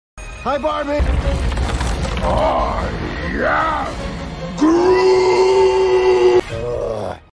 When a girl is mad at you - Mad ken meme vs. I am groot meme
When she`s angry and small things you say drive her really mad.
Girls please don`t be angry with me. I have a meme about boys if you`d like to see it.